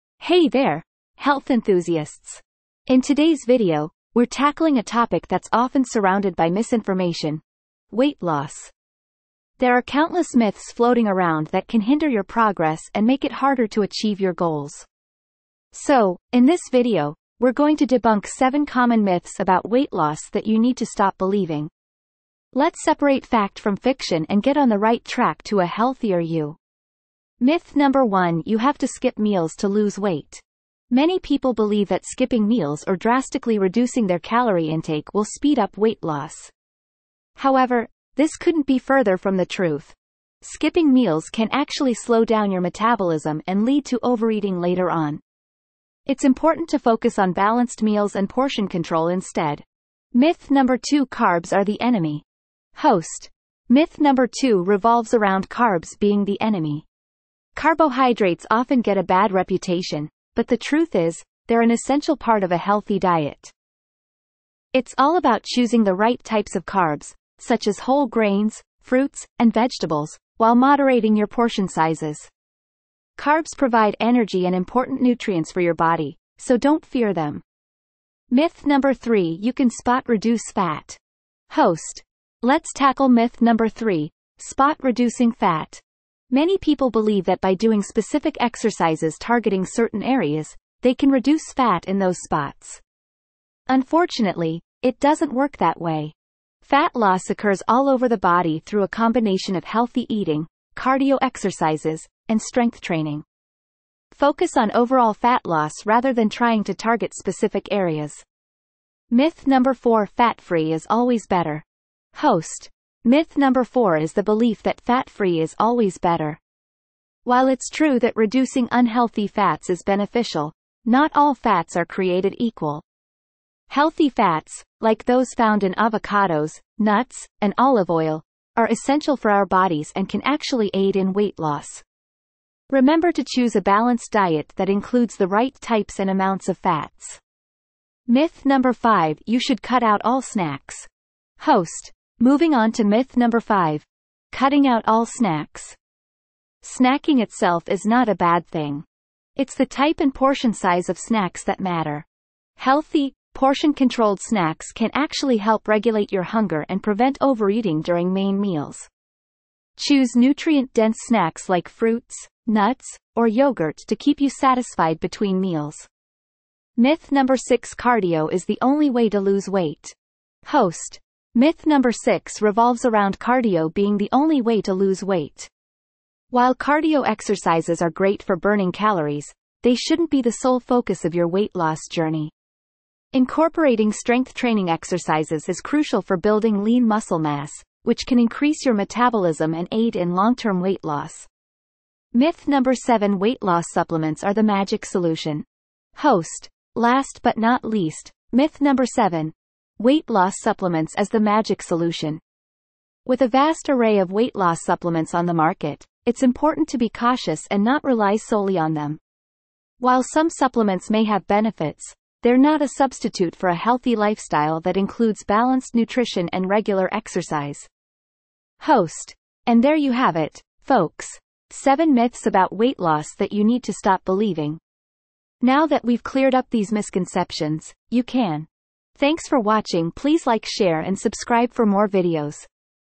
"7 Myths About Weight Loss You Need to Stop Believing"
Discover the truth behind common weight loss myths in 2021! Learn why these myths are holding you back from reaching your goals and what you should actually do for successful weight loss. Stop believing these 7 weight loss myths and start seeing real results now!
In today's video, we're tackling a topic that's often surrounded by misinformation: weight loss. There are countless myths floating around that can hinder your progress and make it harder to achieve your goals. So, in this video, we're going to debunk seven common myths about weight loss that you need to stop believing. Let's separate fact from fiction and get on the right track to a healthier you!